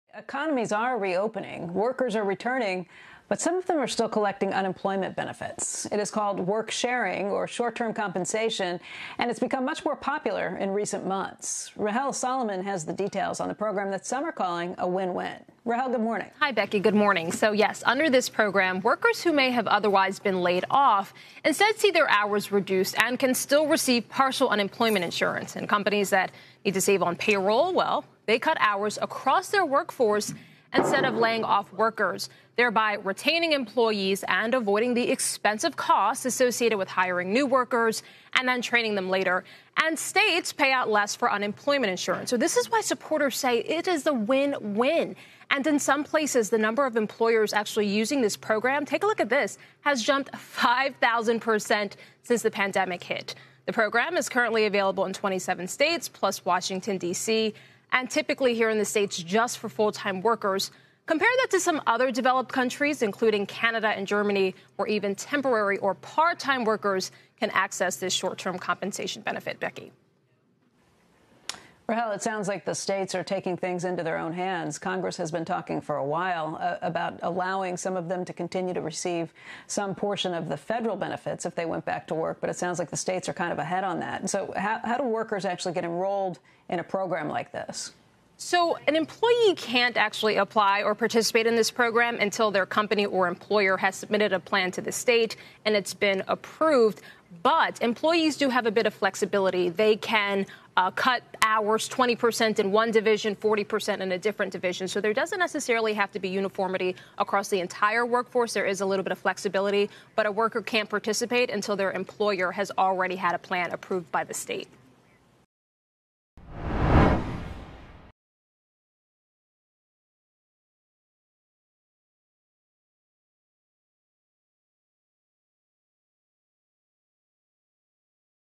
How workers with reduced hours can collect partial unemployment benefits
Employees are returning to work as states begin reopening their economies, but some workers are still collecting unemployment benefits through a program called worksharing or short-term compensation. CNBC's Rahel Solomon reports.